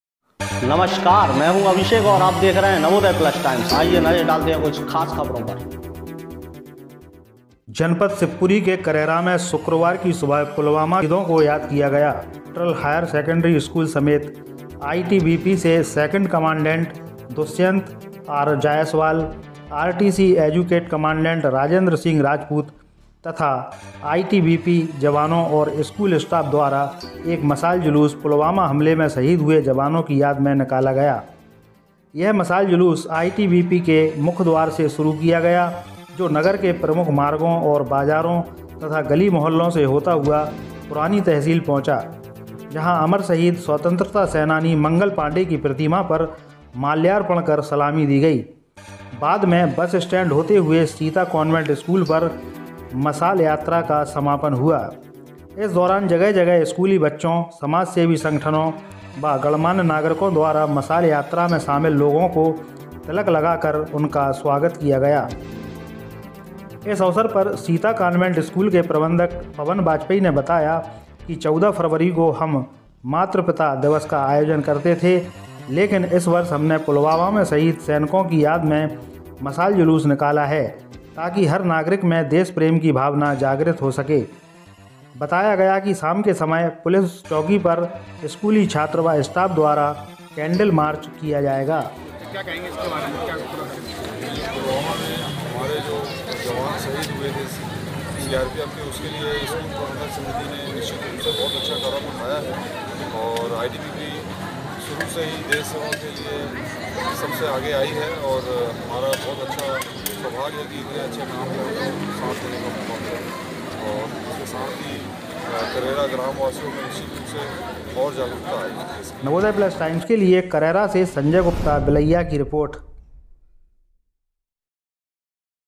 पुलवामा की बरसी पर करैरा में सीता कान्वेंट और आईटीबीपी के जवानों ने निकाला मशाल जुलूस।
रिपोर्ट : संजय गुप्ता (बिलैया) / करैरा (शिवपुरी)